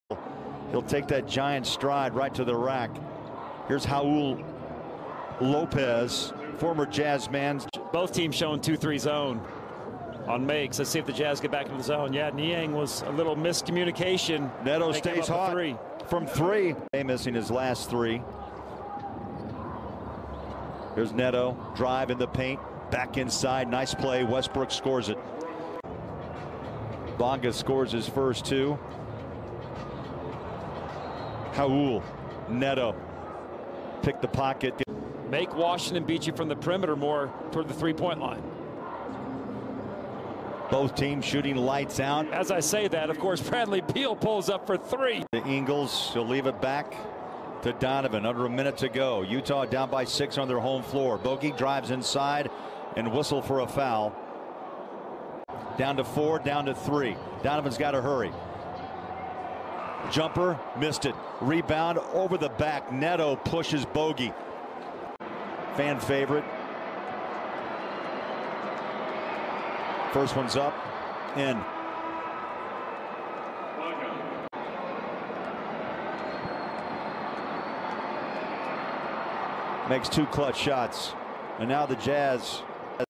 Raul Neto 7 PTS: All Possessions (2021-04-12)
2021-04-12 Utah Jazz vs Washington Wizards: Player Highlights Raul Neto
    7 PTS, 0 REB, 2 AST, 1 STL.
        1 of 1 from 3.
        2 of 2 FGs in 15:02 minutes.